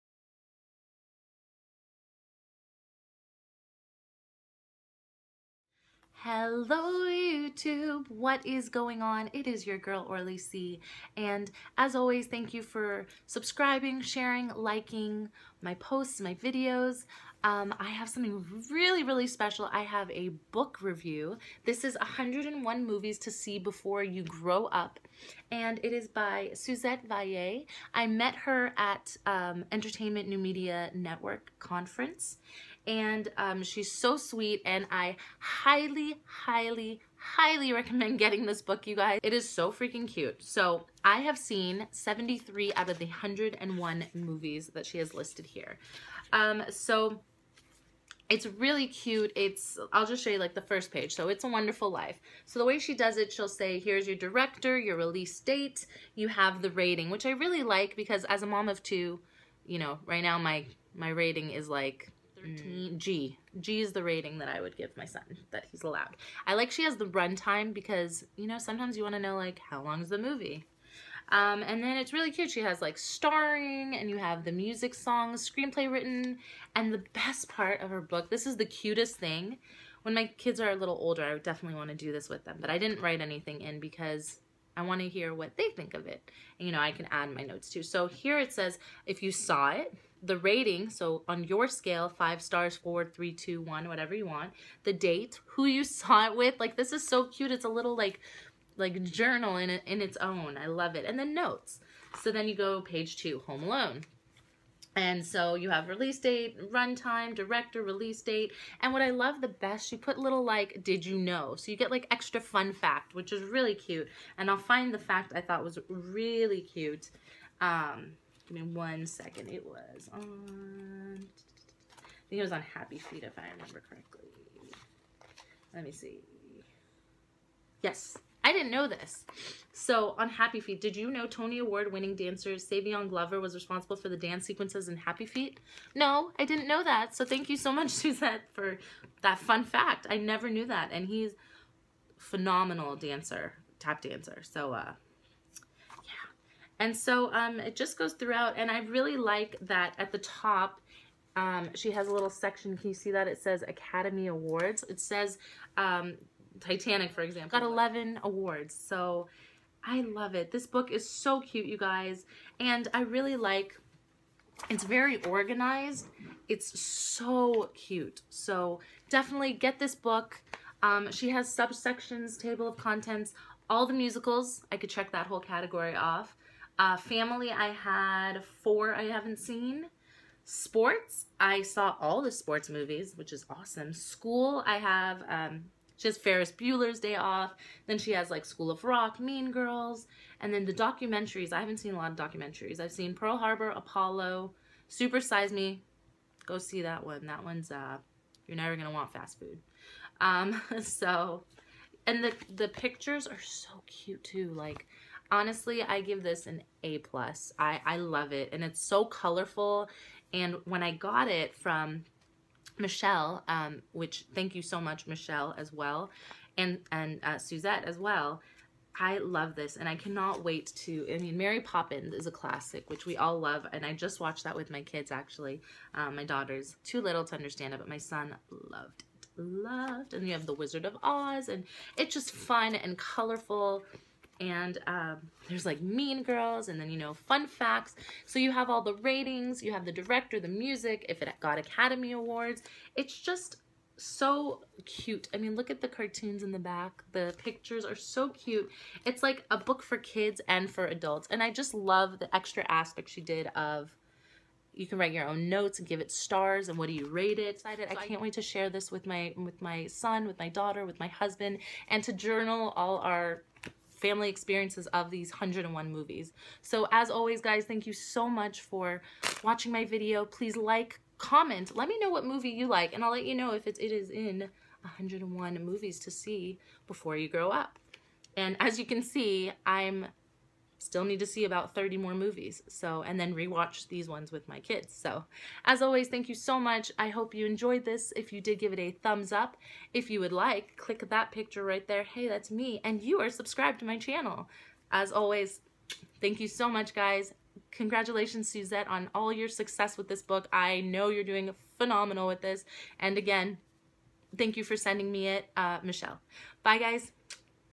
101 Movies to See Before You Grow Up Book Review
I was sent 101 Movies to See Before You Grow Up for review. (Thank you Michelle for sending me this.) It is truly a book for kids and adults of all ages.

If you would like to purchase this book go